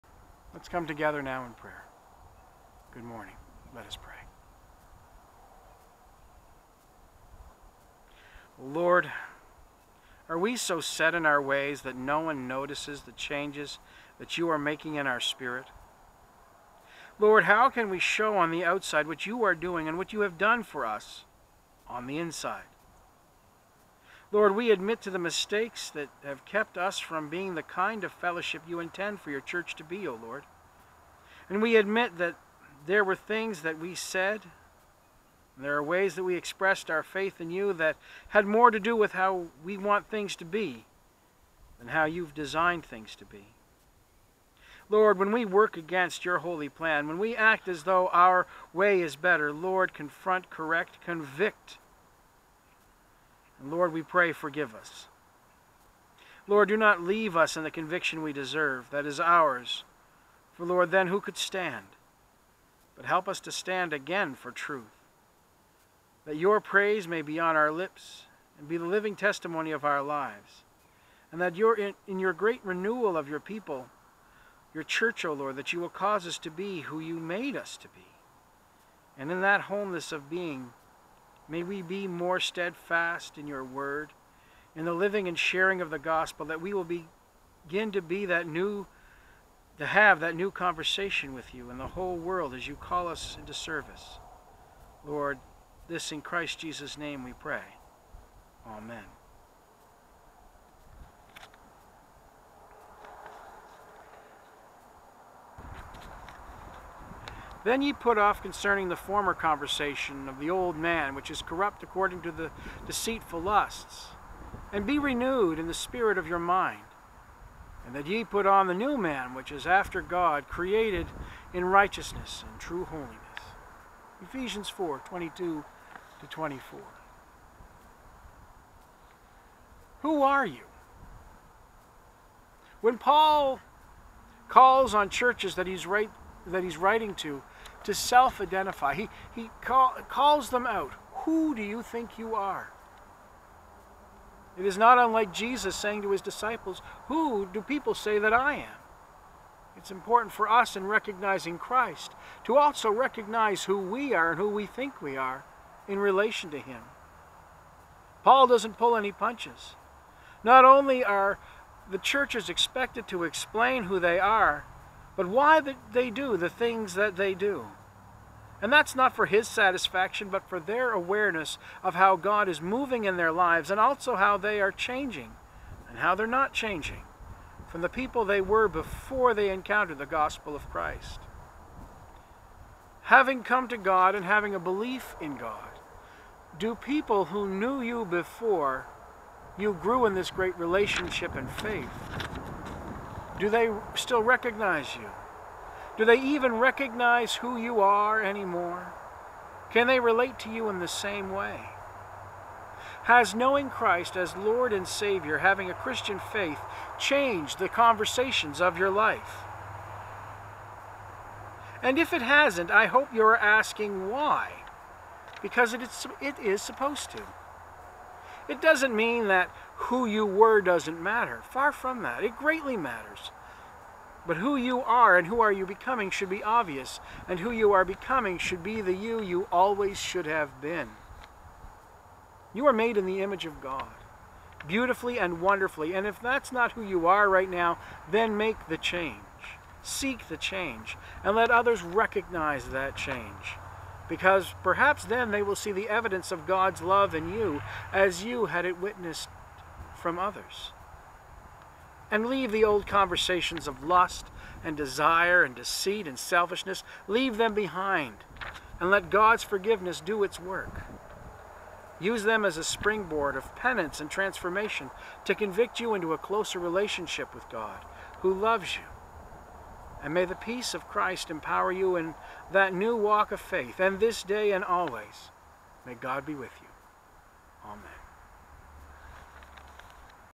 Morning Prayer & Devotions - Ephesians 4:22-24 - May 30 2020
Our identity is something we grow to understand as we continue in the relationship with God. It sometimes means we have to let go with how we see ourselves, or how others have seen us to embrace who we are mean to be in order to glorify God to the fullest of our ability.